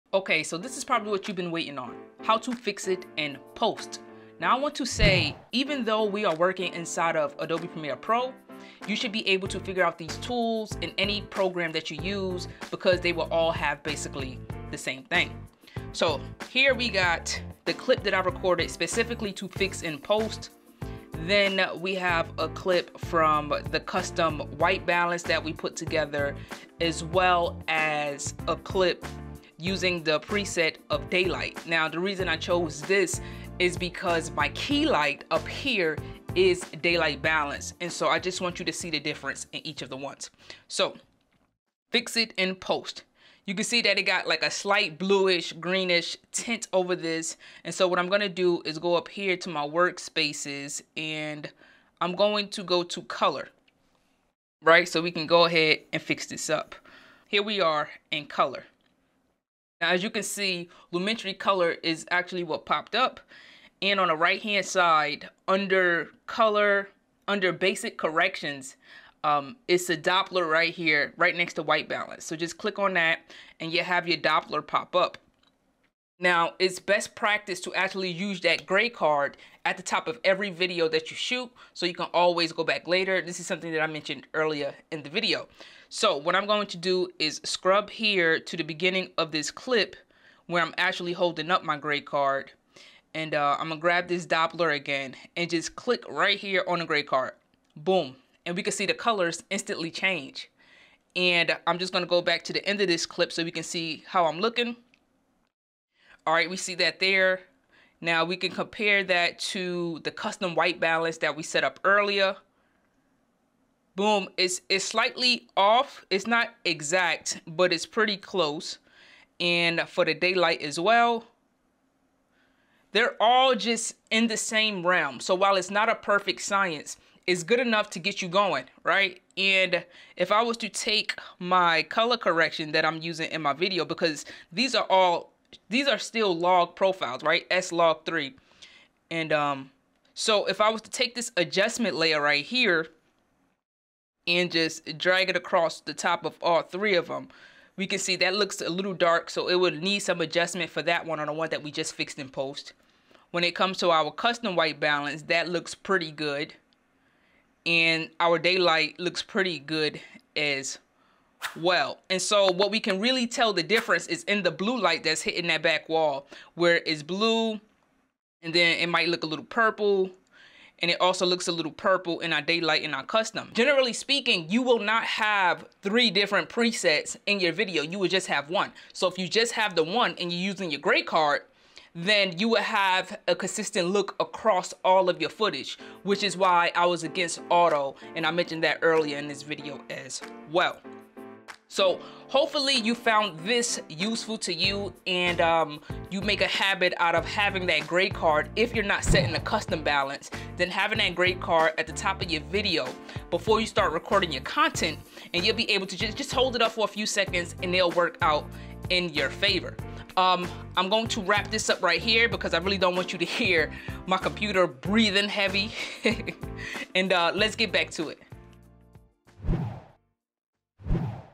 Correct White Balance in Premiere Pro
Using a gray card in your video can make a massive difference when white balancing & "fixing it in post."

Feel free to jump around to the most relevant parts for where you are in your journey.

00:00 -- Intro & Overview
00:17 -- Correcting White Balance in Premiere Pro
03:10 -- Outro

If you know someone else who may also be able to gain insight from this video make sure you send them this link. They'll thank you for thinking of them.

Love.